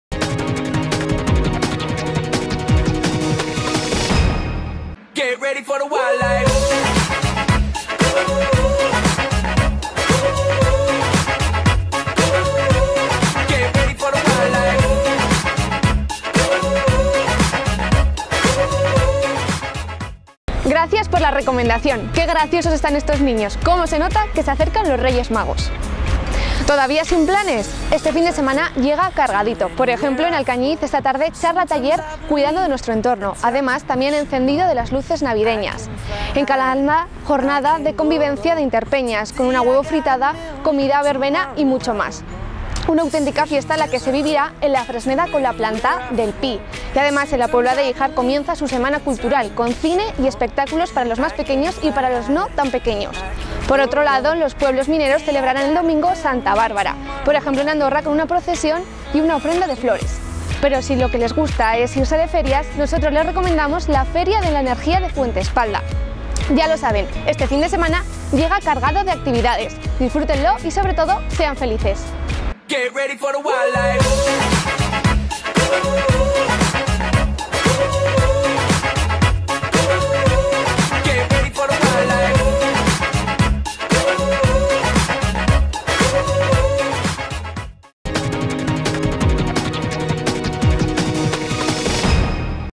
Agenda cultural fin de semana 3 y 4 diciembre
Agenda cultural para el fin de semana del 3 y 4 de diciembre que nos presenta desde Alcañiz, Eva Bielsa, del grupo de comunicación La COMARCA. Destacamos este fin de semana el encendido de luces de Navidad en gran parte del territorio y también la feria de la Energía en Fuentespalda. No te quedes en casa, hay mucho que ver.